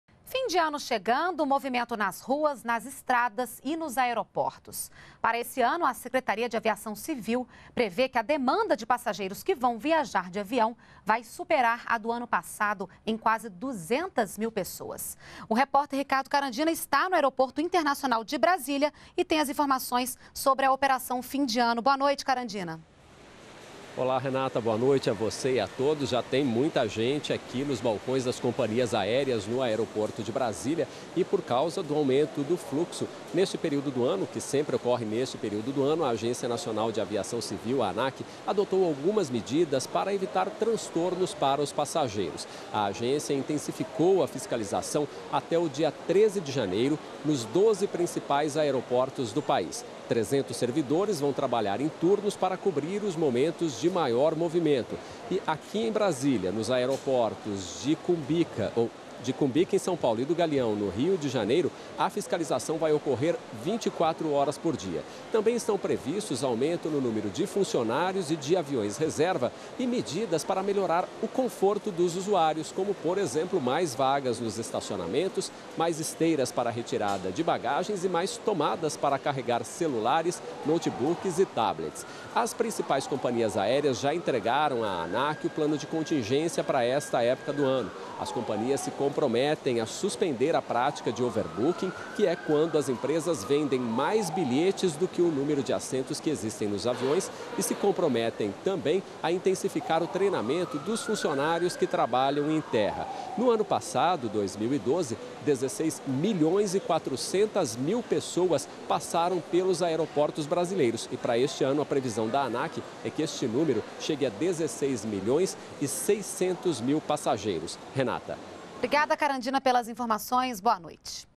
Aeroportos devem receber aumento de 200 mil passageiros no período de fim de ano
NBR NOTÍCIAS - 20.12.13: Fim de ano chegando, movimento nas ruas, nas estradas, nos aeroportos. E para este ano, a Secretaria de Aviação Civil (SAC) prevê que a demanda de passageiros, que vai viajar de avião, vai superar a do ano passado em quase 200 mil pessoas.